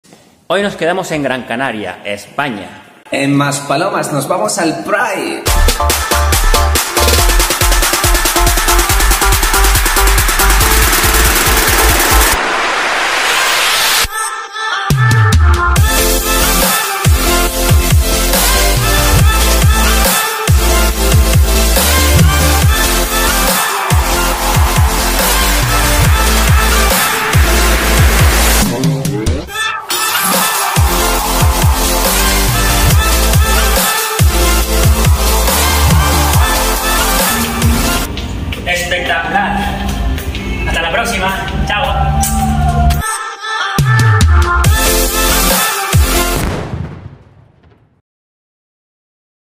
Gus Jackson FREEDOM MASPALOMAS PRIDE FESTIVAL 2022 Michael Jackson Tribute
Resumen de una super noche con sabor a JACKSON!